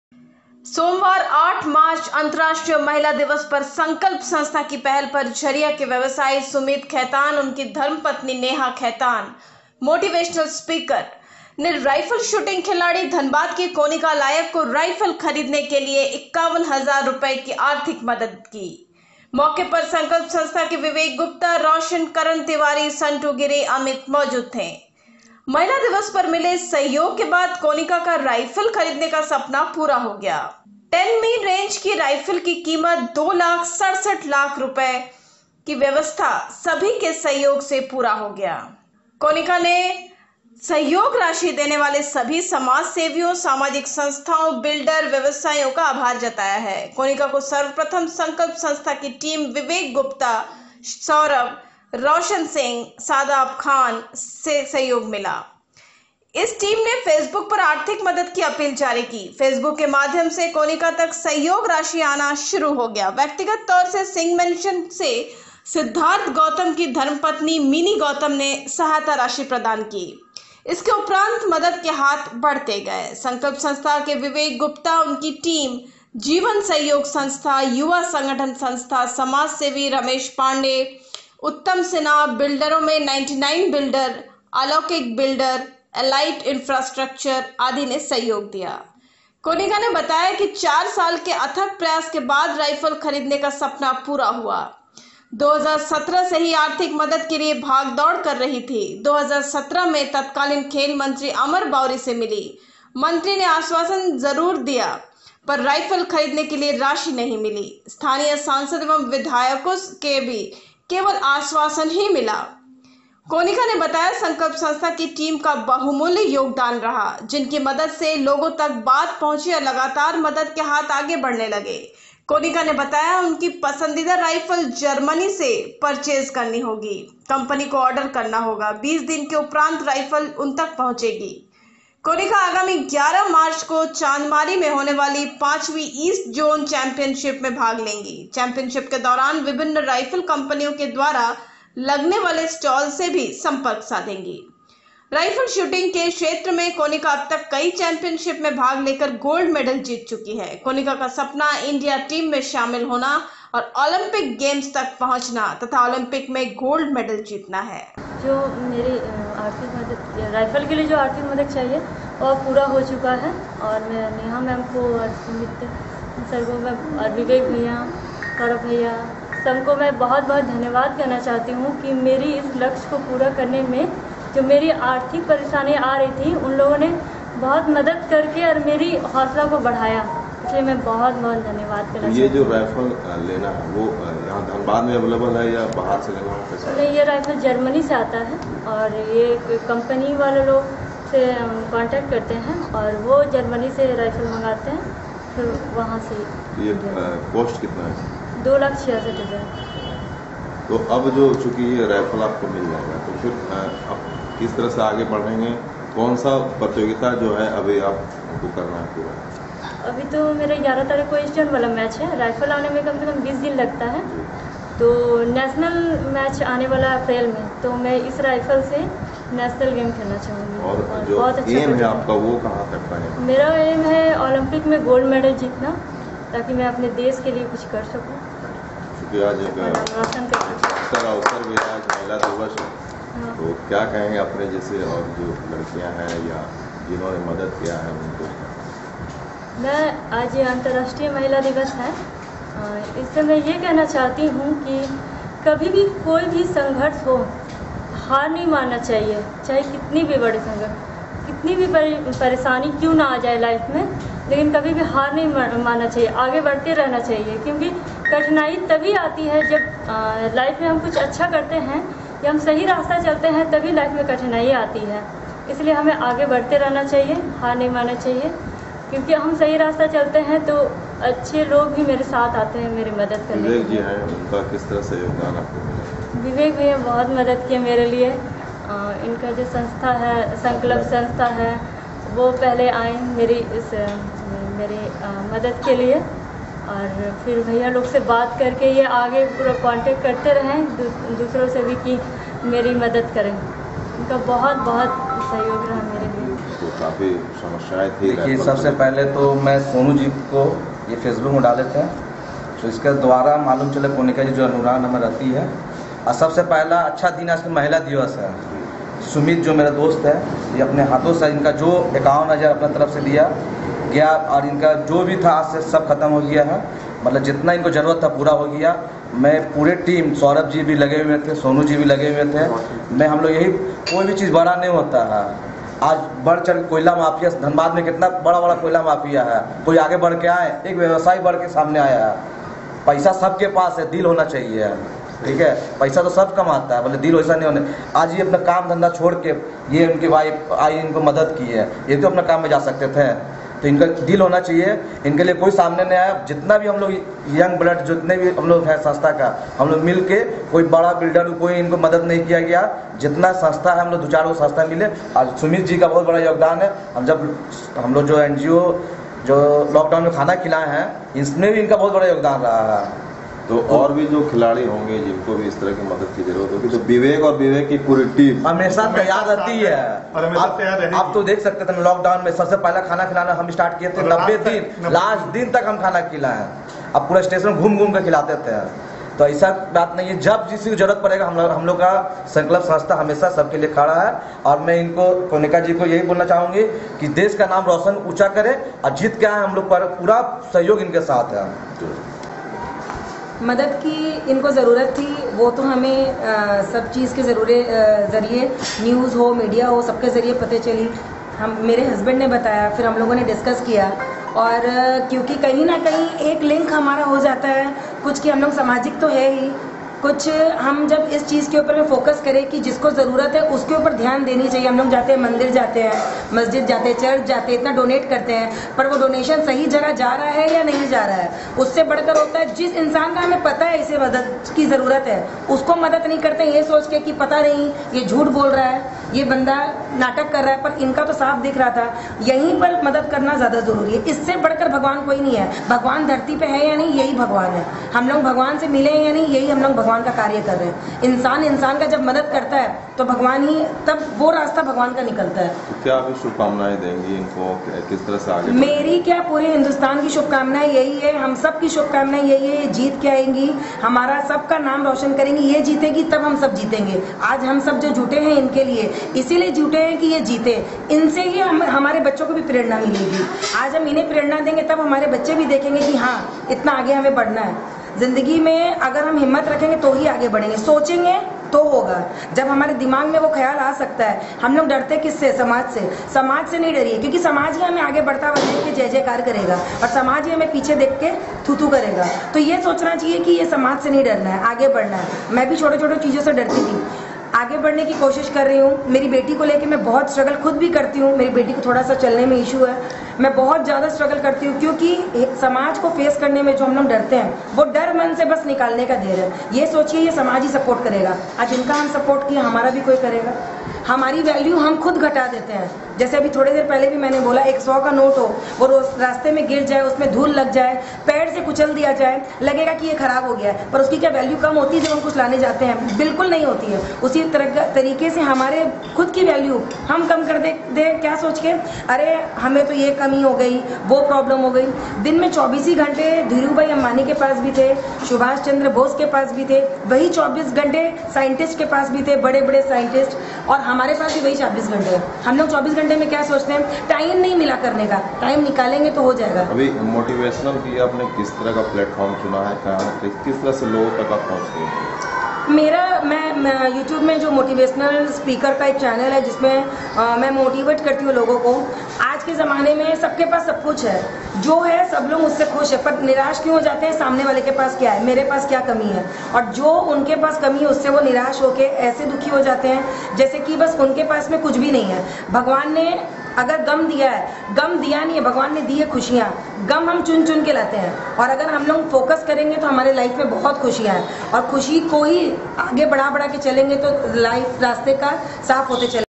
Aaj ka sach news,,08.03.2021
धनबाद। सोमवार 8 मार्च अंतराष्ट्रीय महिला दिवस पर संकल्प संस्था की पहल पर झरिया के व्यवसायी सुमित खेतान उनकी धर्मपत्नी नेहा खेतान (मोटिवेशनल स्पीकर) ने
राइफल शूटिंग खिलाड़ी धनबाद की कोनिका लायक को राइफल खरीद के लिए 51 हजार रु की आर्थिक मदद दी। मौके पर संकल्प संस्था से विवेक गुप्ता , रौशन , करण तिवारी , संटू गिरी , अमित मौजूद थे। महिला दिवस पर मिले इस सहयोग के बाद कोनिका का राइफल खरीद का सपना पूरा हो गया है। 10 मी रेंज की राइफल की कीमत 2.66 लाख रु की व्यवस्था सभी के सहयोग पूरा हो गया। कोनिका ने सहयोग राशि देने वाले सभी समाज सेवियो , सामाजिक संस्थाओं , बिल्डर , व्यवसायियों का आभार जताया है। कोनिका को सर्वप्रथम संकल्प संस्था की टीम विवेक गुप्ता , सौरभ , रौशन सिंह , सदाब खान का सहयोग मिला। इस टीम ने फेसबुक पर आर्थिक मदद की अपील जारी की। फेसबुक के माध्यम से कोनिका तक सहयोग राशि आना शुरू हो गया। व्यक्तिगत तौर से सिंह मेंशन से सिद्धार्थ गौतम की धर्मपत्नी मिनी गौतम ने सहायता राशि प्रदान की। इसके उपरांत मदद के हाथ बढ़ते गए। संकल्प संस्था विवेक गुप्ता उनकी टीम , जीवन सहयोग संस्था , युवा संगठन संस्था , समाजसेवी रमेश पांडेय , उत्तम सिन्हा , बिल्डरों में 99 बिल्डर , अलौकिक बिल्डर , एलाइट इंफ्रास्ट्रक्चर आदि ने सहयोग दिया। कोनिका ने बताया चार साल के अथक प्रयास के बाद राइफल खरीद का सपना पूरा हुआ। 2017 से ही आर्थिक मदद के लिए भागदौड़ कर रही थी। 2017 में तत्कालीन खेल मंत्री अमर बाउरी से मिली। मंत्री से आश्वासन जरूर मिला पर राइफल खरीद के लिए राशि नही मिली। स्थानीय सांसद व विधायकों से भी केवल आश्वासन ही मिला। कोनिका ने बताया संकल्प संस्था की टीम का बहुमूल्य योगदान रहा। जिनकी मदद से लोगों तक बात पहुँची और लगातार मदद के हाथ आगे बढ़ने लगे। कोनिका ने बताया उनकी पसंदीदा राइफल जर्मनी से पर्चेज करनी होगी। कंपनी को ऑर्डर करना होगा। 20 दिन के उपरांत  राइफल उनतक पहुँचेगी।
कोनिका आगामी 11 मार्च को चांदमारी में होने वाली पांचवी ईस्ट ज़ोन चैम्पियंशिप में भाग लेंगी। चैम्पियंशिप के दौरान विभिन्न राइफल कंपनियों के द्वारा लगनेवाले स्टॉल से भी सम्पर्क साधेंगी। राइफल शूटिंग के क्षेत्र में कोनिका अबतक कई चैम्पियंशिप में भाग लेकर गोल्ड मेडल जीत चुकी है। कोनिका का सपना इंडिया टीम में शामिल होना और ओलंपिक गेम्स तक पहुचना तथा ओलंपिक में गोल्ड मेडल जितना है।